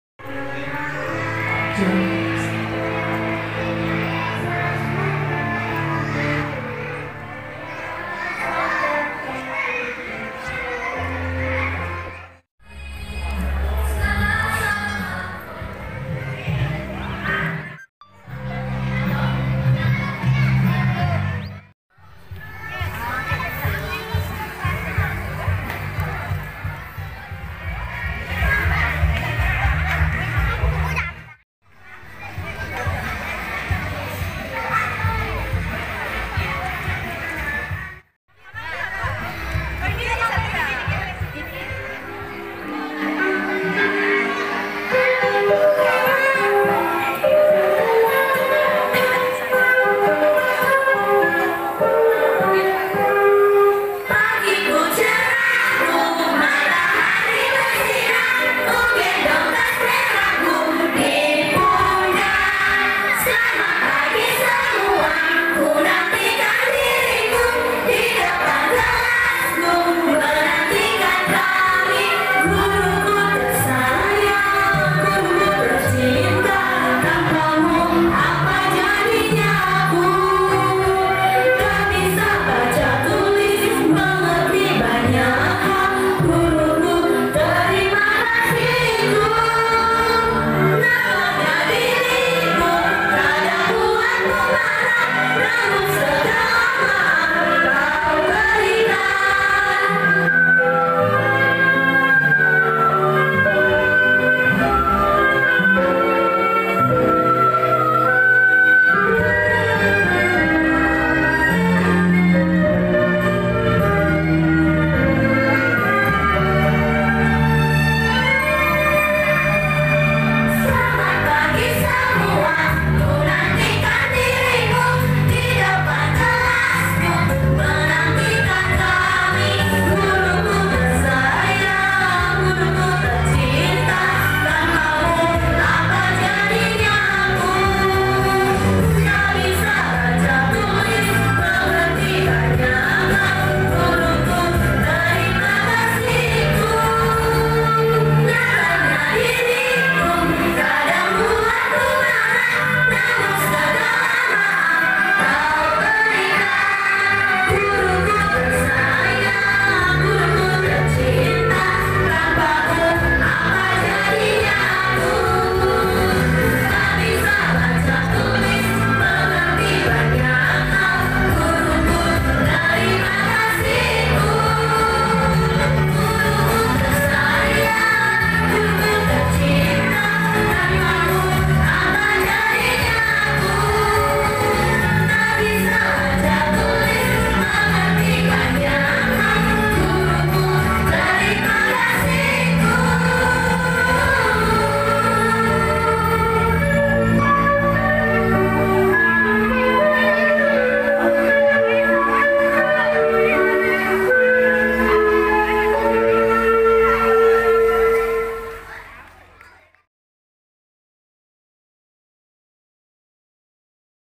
Anak Tk bergembira bernyanyi,bersama ibu gurunya
Tk Duta Kusuma Pertiwi besukaria bernyanyi.